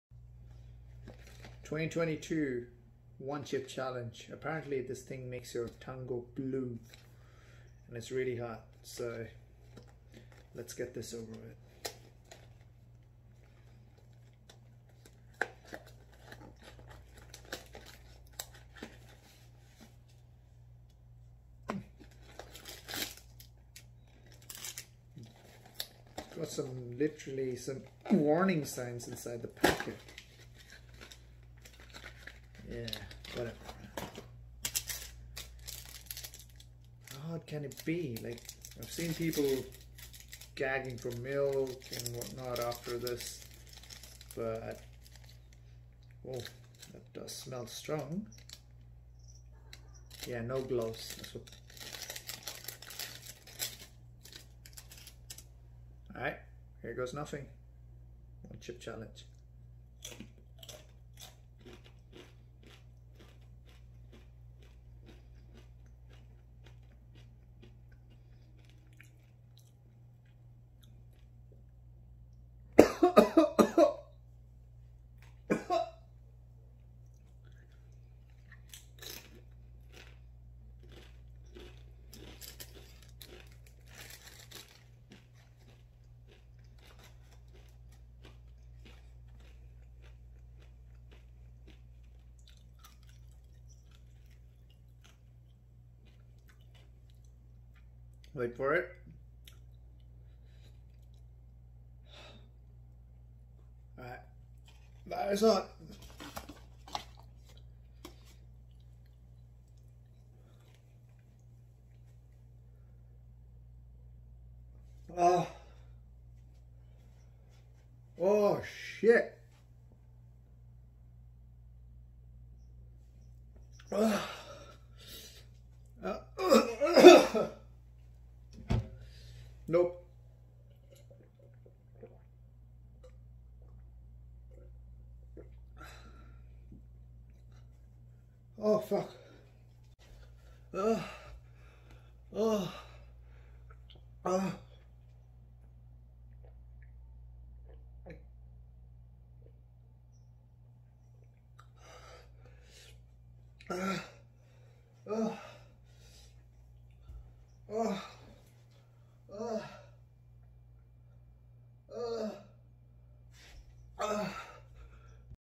Nearly fainted after the PAQUI ONE CHIP, NEVER EVER AGAIN!!!
Hot is an understatement, I couldn't stand, talk, even drink properly for more than 30 minutes. Never again!